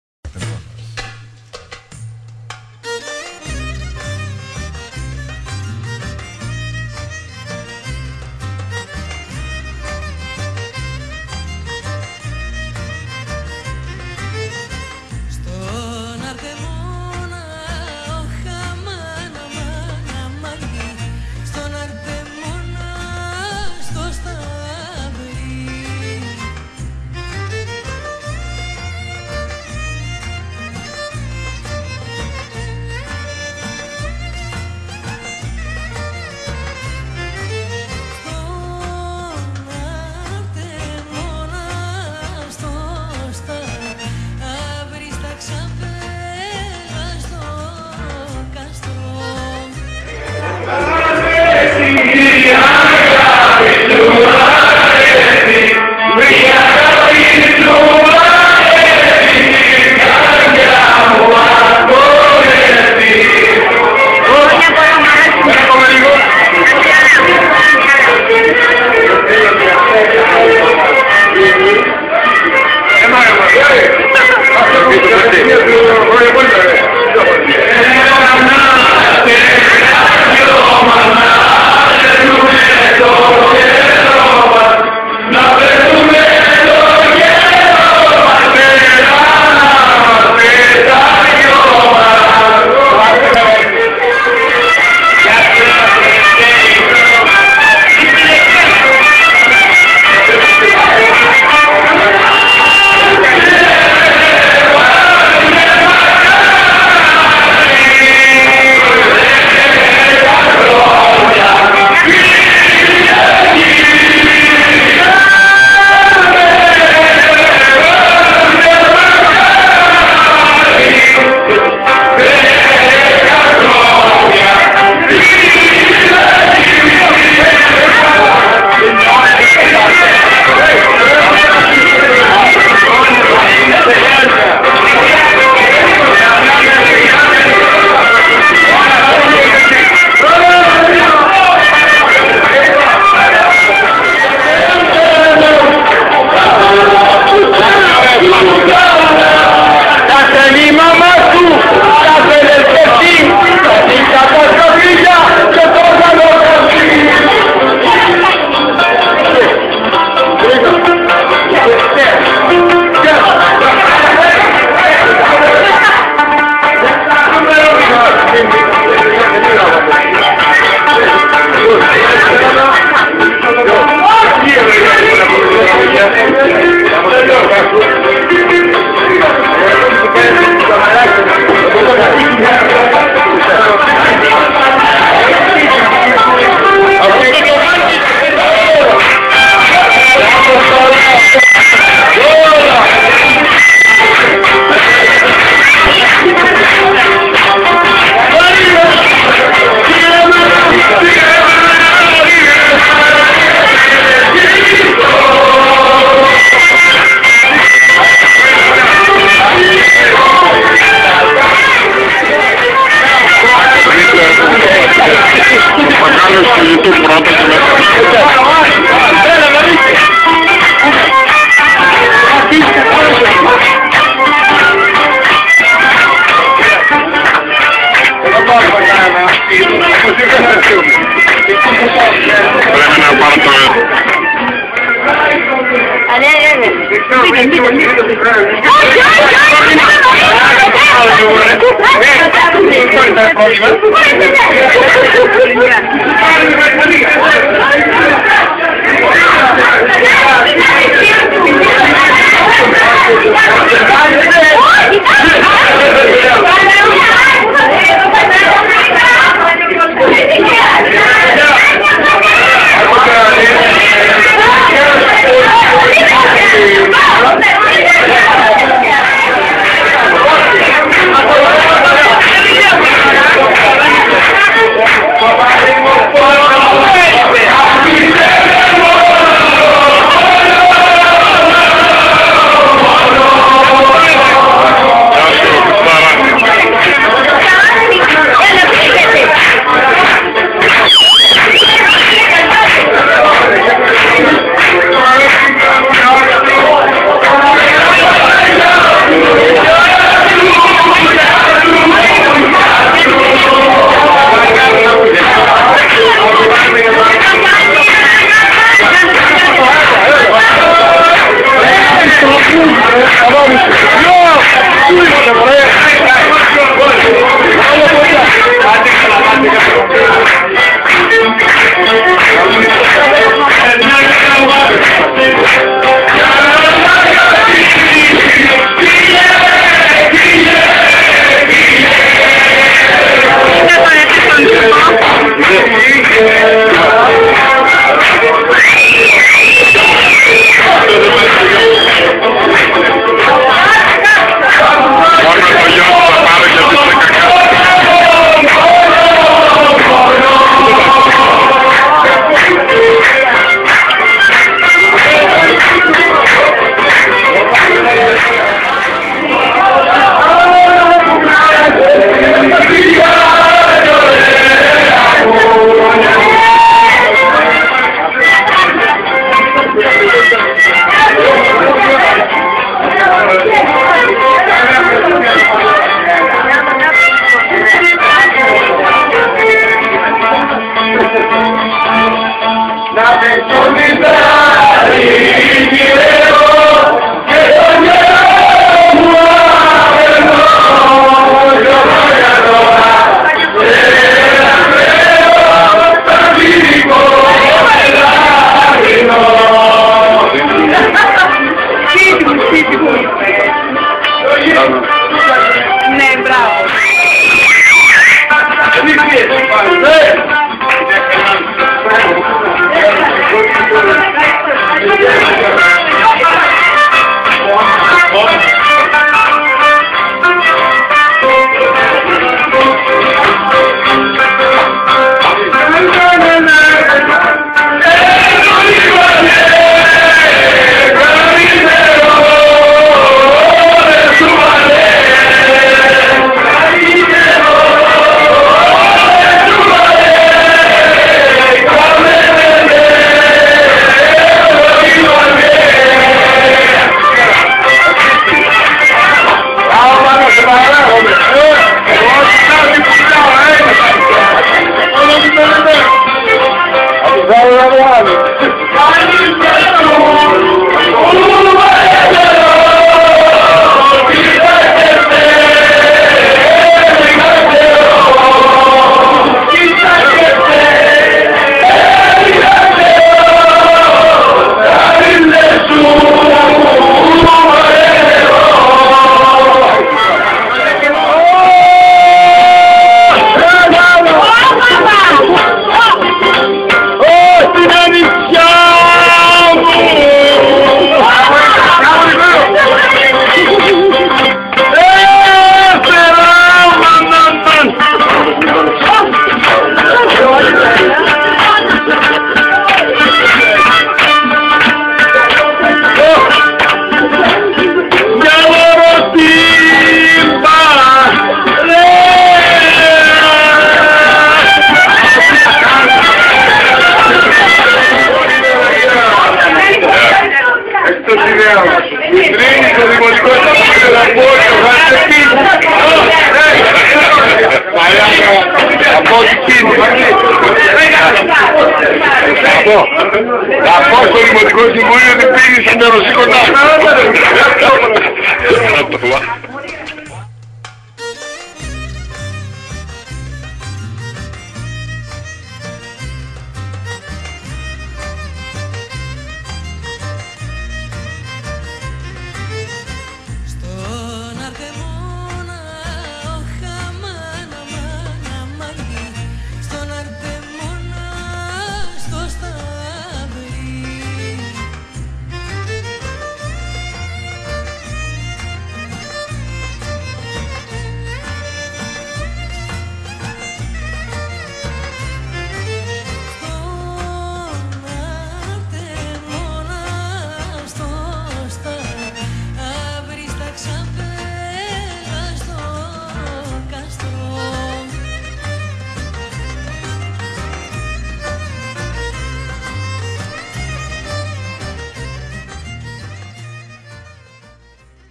Sifnos - Αρτεμωνιάτικο γλέντι
21 Νοεμβρίου της Παναγίας οι Αρτεμωνιάτες τήρησαν το έθιμο μετά την εκκλησία σε καφενείο της γειτονιάς και μετά βόλτα με τακίμι για βίζιτες (Γιορτές)